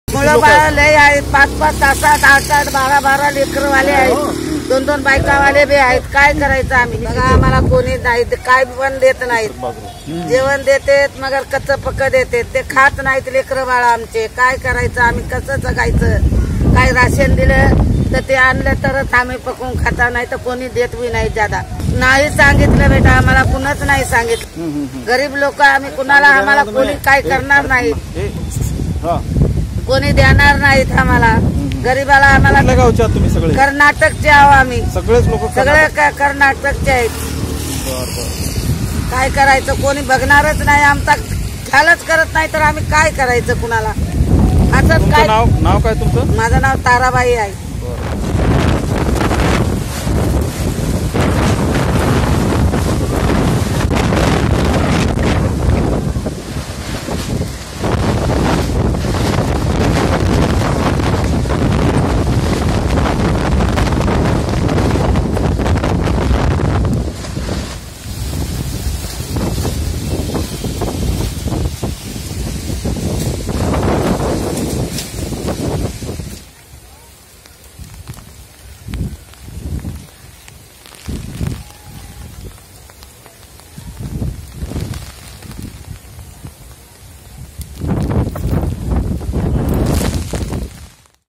मुंबई: घरात लेकरं बाळ आहेत, आम्हाला या वादळा बद्दल कुणीच सांगीतलेलं नाही
मुंबईमध्ये काही मिनिटांच्या अंतरावर निसर्ग वादळ येऊन धडकण्याची शक्यता असतानाच मानखुर्द मध्ये असलेल्या महाराष्ट्र नगर, मंडला गाव, चिता कॅम्प, साठे नगर, परिसरातील अनेक लोक पालात आणि कच्च्या झोपड्यात राहतात. या लोकांना प्रशासनानं कोणतीही माहिती दिली नसल्यामुळं या लोकांना या वादळाबाबत कुठलीही माहिती नव्हती.